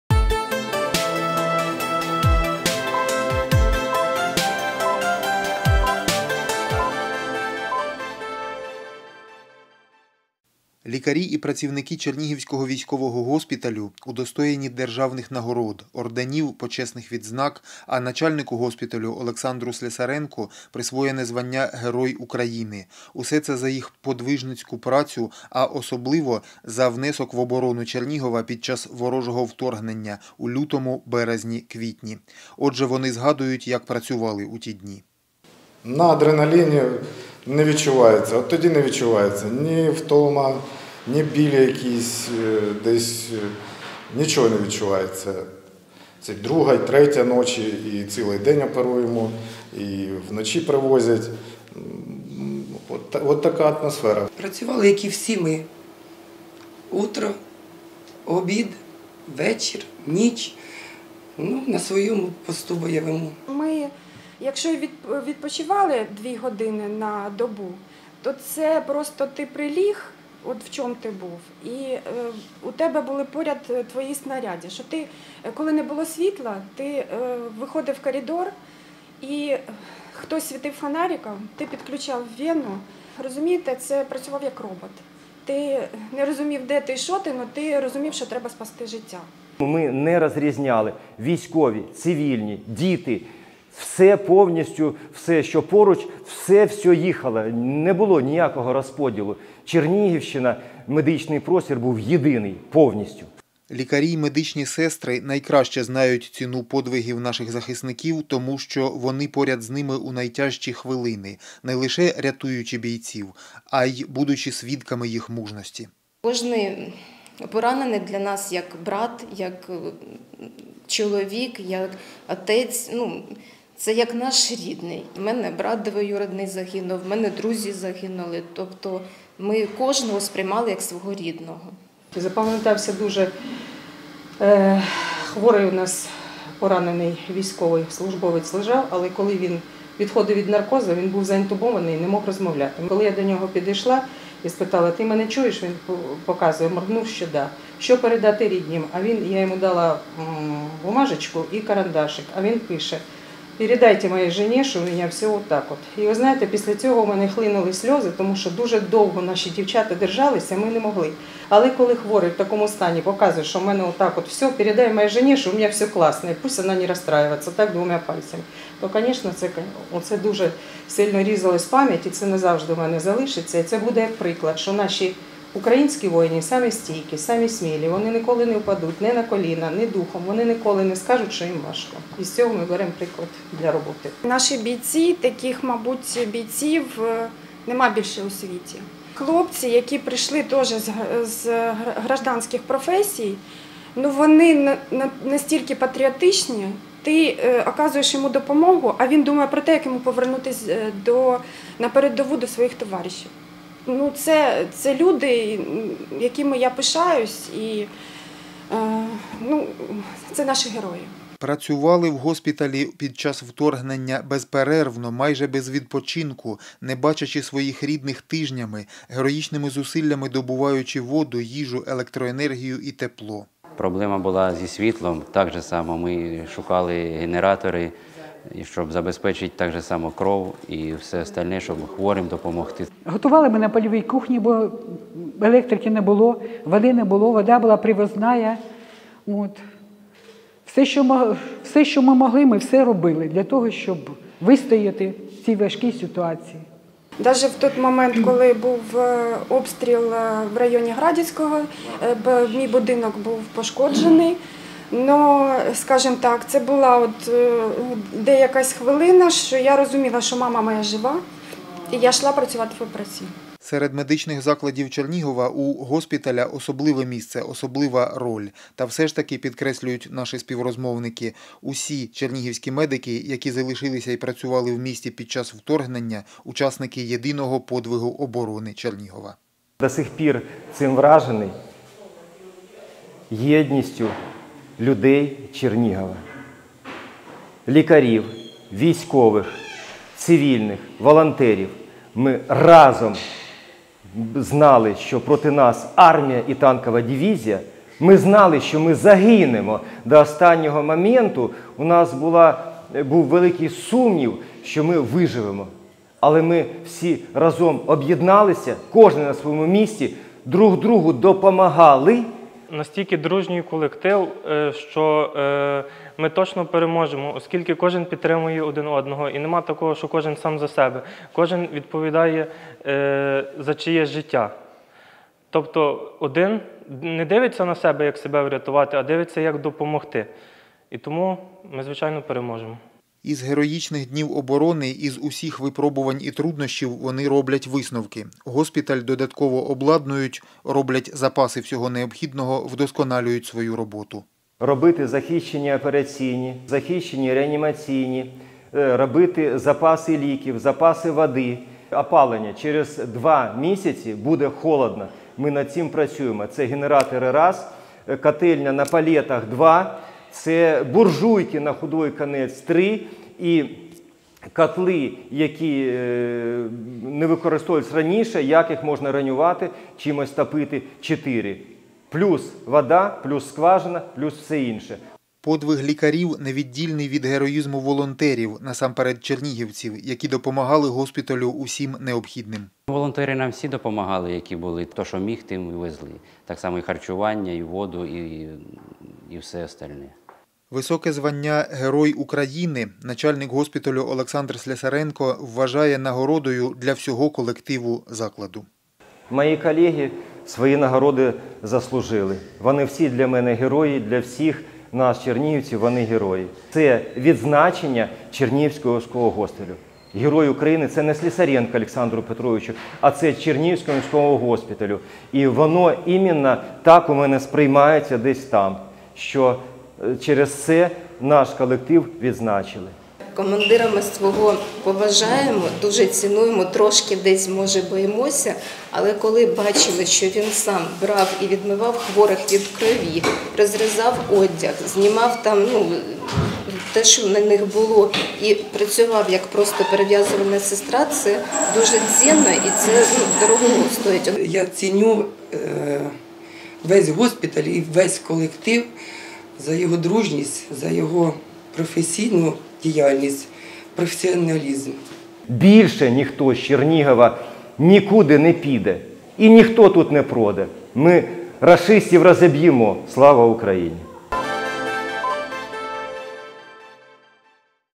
Лікарі Чернігівського військового госпіталю відзначені державними нагородами .
Лікарі Чернігівського військового госпіталю відзначені державними нагородами за особливий внесок в оборону міста та області під час ворожого вторнення. Начальнику госпіталю Олександру Слєсаренку присвоєне звання Герой України; сам він вважає цю нагороду визнанням подвигу й праці всього колективу.